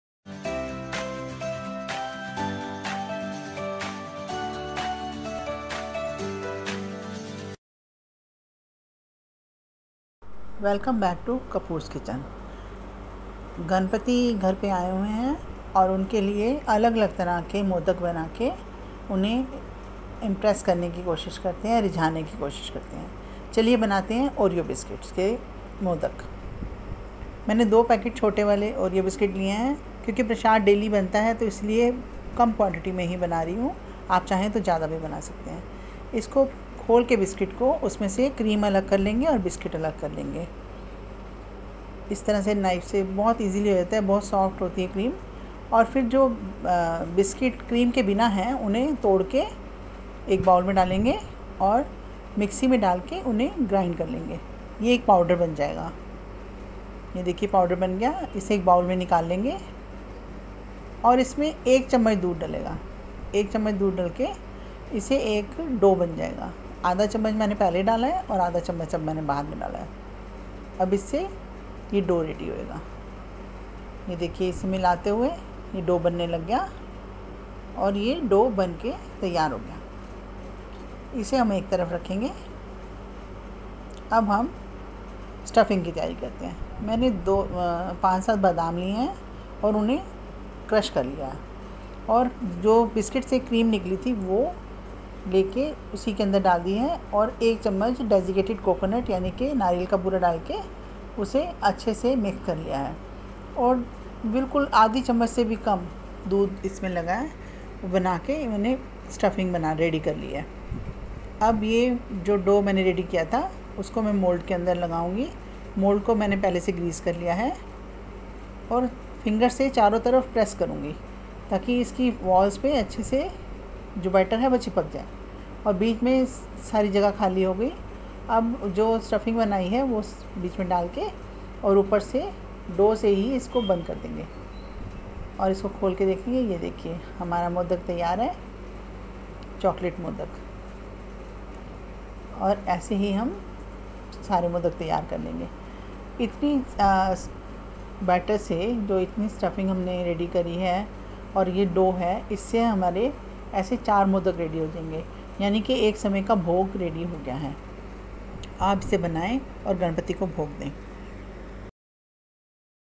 Ganesh Chaturthi Special Chocolate Modak | Easiest Recipe | Cook with Simmi | Simmi Kapoor
It is very tasty and easy and also very quick recipe
Required Ingredients are :-
1 Oreo Biscuits
2 Desiccated Coconut
3 Almond
4 Milk
5 Modak Mould